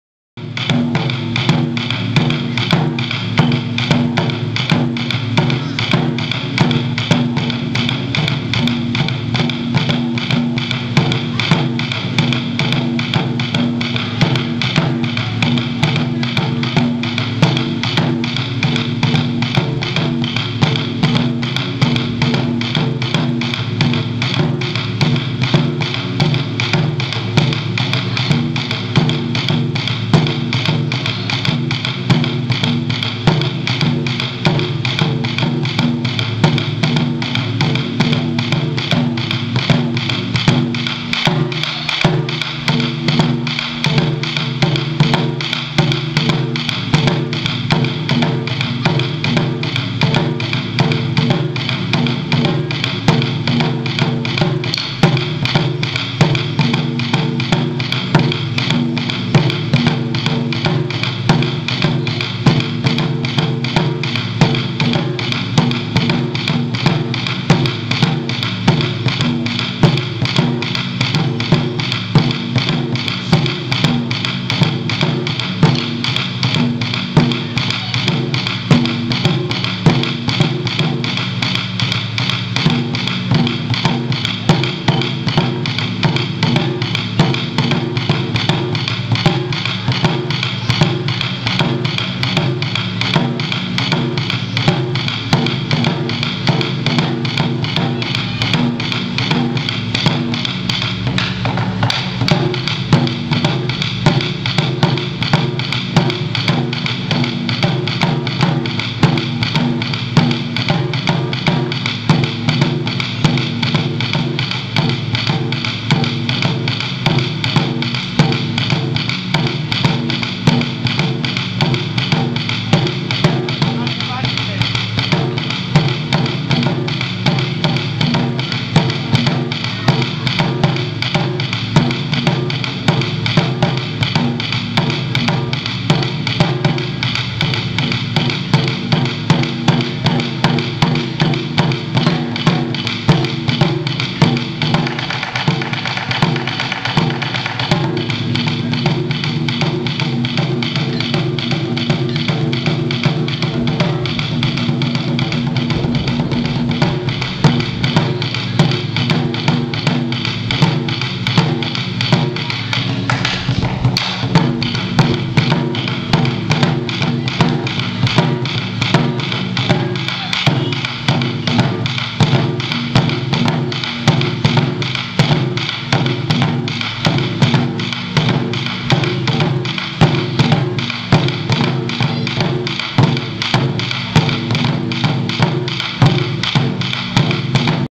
Rangetsu Drums
This is a snippet of the drum show at Rangetsu in Orlando, Florida.